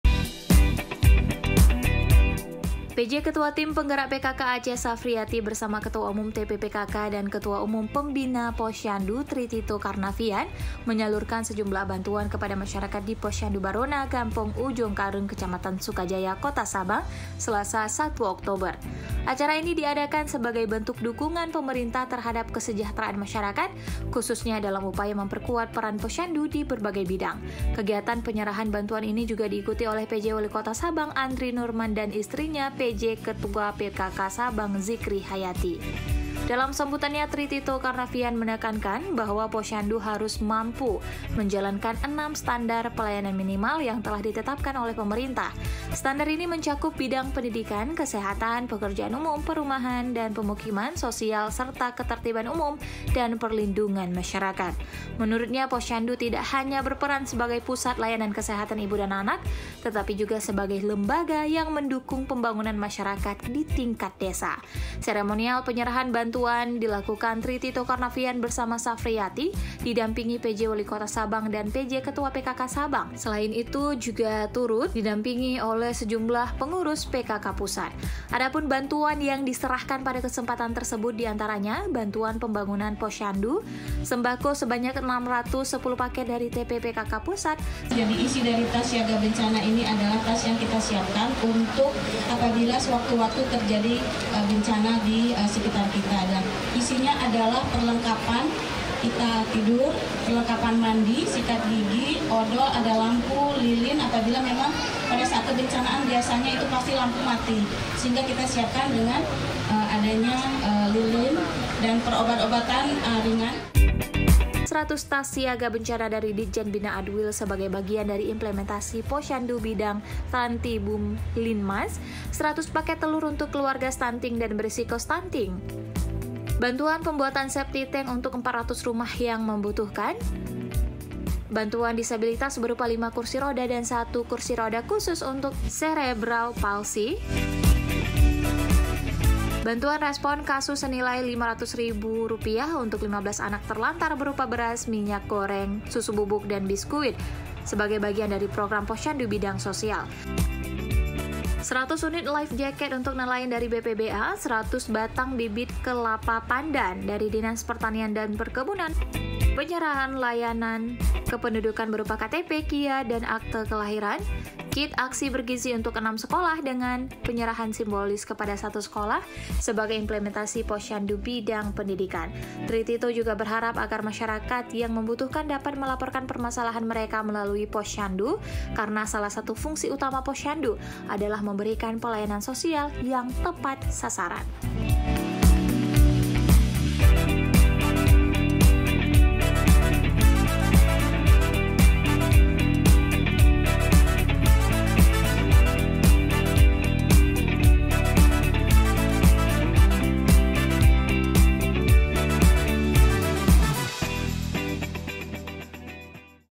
Perkuat Peran Posyandu, Pj Ketua PKK Aceh dan Ketua Umum PKK Salurkan Bantuan untuk Masyarakat Sab
SABANG - Pj Ketua Tim Penggerak (TP) PKK Aceh, Safriati, bersama Ketua Umum TP PKK dan Ketua Umum Pembina Posyandu, Tri Tito Karnavian, menyalurkan sejumlah bantuan kepada masyarakat di Posyandu Baroena, Gampong Ujong Kareung, Kecamatan Sukajaya, Kota Sabang, Selasa (1/10). Acara ini diadakan sebagai bentuk dukungan pemerintah terhadap kesejahteraan masyarakat, khususnya dalam upaya memperkuat peran posyandu di berbagai bidang.

Kegiatan penyerahan bantuan ini juga diikuti oleh Pj Walikota Sabang Andri Nourman dan istrinya, Pj Ketua PKK Sabang Zikri Hayati.

Dalam sambutannya, Tri Tito Karnavian menekankan bahwa Posyandu harus mampu menjalankan 6 Standar Pelayanan Minimal (SPM) yang telah ditetapkan oleh pemerintah. Standar ini mencakup bidang pendidikan, kesehatan, pekerjaan umum, perumahan dan pemukiman, sosial, serta ketertiban umum dan perlindungan masyarakat. Menurutnya, posyandu tidak hanya berperan sebagai pusat layanan kesehatan ibu dan anak, tetapi juga sebagai lembaga yang mendukung pembangunan masyarakat di tingkat desa.

"Posyandu adalah lembaga penting di desa yang berfungsi untuk mendata dan menyelesaikan masalah-masalah warga. Harapannya, posyandu bisa terus bekerja sama dengan berbagai pihak, mulai dari pemerintah desa hingga kabupaten, sehingga pelayanan yang diberikan lebih efektif," ujar Tri Tito Karnavian.

Seremonial penyerahan bantuan dilakukan Tri Tito Karnavian bersama Safriati didampingi Pj Walikota Sabang dan Pj Ketua PKK Sabang. Selain itu juga turut didamping oleh sejumlah pengurus PKK Pusat.

Adapun bantuan yang diserahkan pada kesempatan tersebut di antaranya:

- Bantuan pembangunan Posyandu (Banper).
- Sembako sebanyak 610 paket dari TP PKK Pusat.
- 100 Tas Siaga Bencana dari Ditjen Bina Adwil, sebagai bagian dari implementasi Posyandu bidang Trantibumlinmas.
- 100 paket telur untuk keluarga stunting dan berisiko stunting, sebagai implementasi Posyandu bidang kesehatan.
- Bantuan pembuatan septik tank untuk 400 rumah yang membutuhkan, terkait program Posyandu bidang perumahan rakyat.
- Bantuan disabilitas berupa 5 kursi roda dan 1 kursi roda khusus untuk cerebral palsy, sebagai implementasi Posyandu bidang sosial.
- Bantuan respon kasus senilai Rp500.000 untuk 15 anak terlantar, berupa beras, minyak goreng, susu bubuk, dan biskuit, sebagai bagian dari program Posyandu bidang sosial.
- 100 unit Life Jacket untuk nelayan dari BPBA, bagian dari implementasi Posyandu bidang Trantibumlinmas.
- 100 batang bibit kelapa pandan dari Dinas Pertanian dan Perkebunan.
- Penyerahan layanan kependudukan berupa KTP, KIA, dan akta kelahiran.
- Kit Aksi Bergizi untuk enam sekolah, dengan penyerahan simbolis kepada satu sekolah, sebagai implementasi Posyandu bidang pendidikan.

Tri Tito juga berharap agar masyarakat yang membutuhkan dapat melaporkan permasalahan mereka melalui posyandu, karena salah satu fungsi utama posyandu adalah memberikan pelayanan sosial yang tepat sasaran.

"Mudah-mudahan bantuan ini dapat bermanfaat bagi yang membutuhkan, dan posyandu terus memainkan perannya dalam mendukung kesejahteraan masyarakat," tutupnya. [